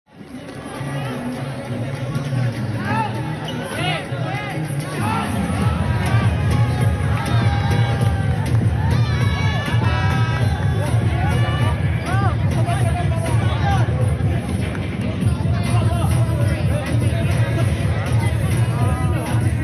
Bay Area forward Zhu Songwei was sent out of the court mid third quarter
LOOK: Bay Area forward Zhu Songwei was sent out of the court mid third quarter.

Viber: Spin_ph